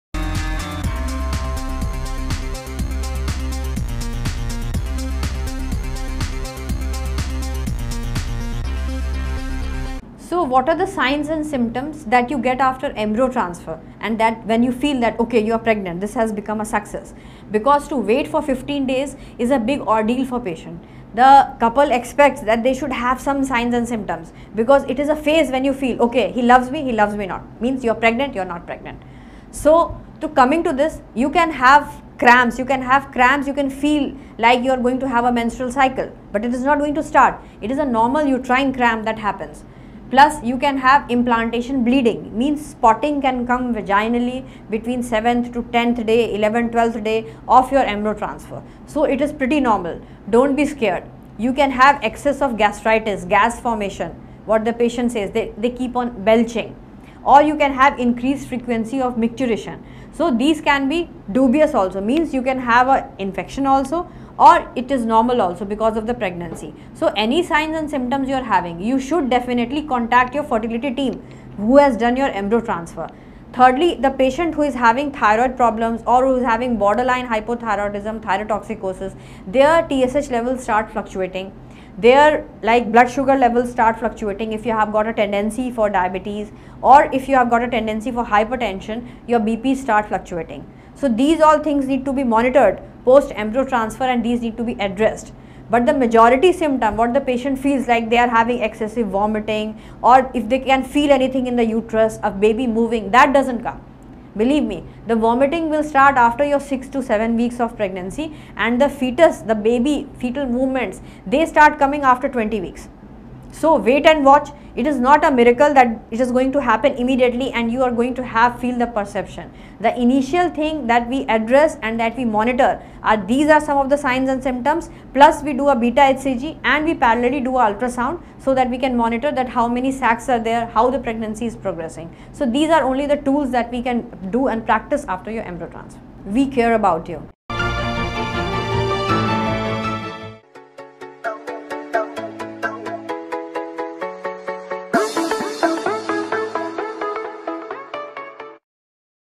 symptoms of pregnancy after ivf embryo transfer| Dr. Richika Sahay Shukla | India IVF
After undergoing in vitro fertilization (IVF) and embryo transfer, it is essential to be aware of potential pregnancy symptoms. While these symptoms can vary from woman to woman, here is a general description of common signs that indicate a pregnancy after IVF embryo transfer:

1- Implantation bleeding: Some women may experience light spotting or minimal vaginal bleeding around 10 to 14 days after embryo transfer. This is caused by the embryo implanting itself into the uterine lining.

2- Breast tenderness: The breasts may become sensitive, tender, or swollen due to hormonal changes associated with pregnancy.

3- Fatigue: Feeling excessively tired or experiencing a sudden drop in energy levels is a common symptom of early pregnancy.

4- Nausea and morning sickness: Many pregnant women experience nausea, particularly in the morning. This can range from mild queasiness to persistent vomiting.

5- Increased urination: The frequency of urination may increase due to hormonal changes and increased blood flow to the kidneys.